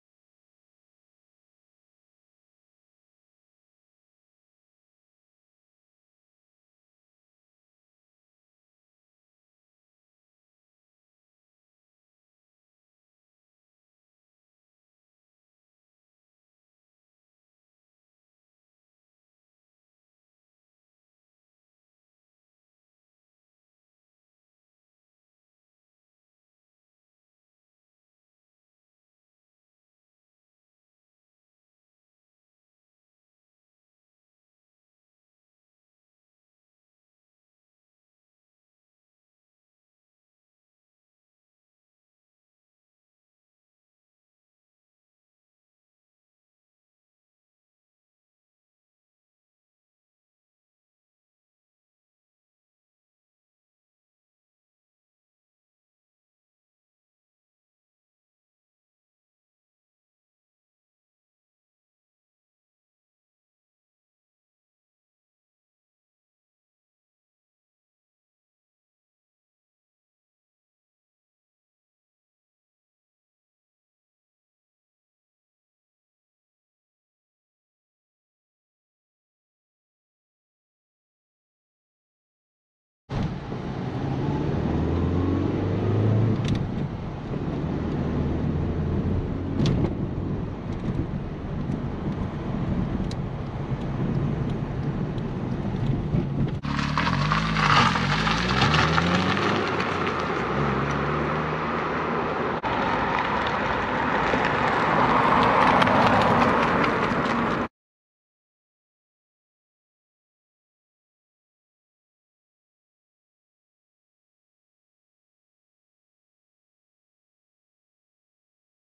1991 Toyota Celica 2.0
Anglia Car Auctions Kings Lynn are pleased to offer this 1991 Toyota Celica 2.0 as an entry in our January 29th and 30th classic car auction.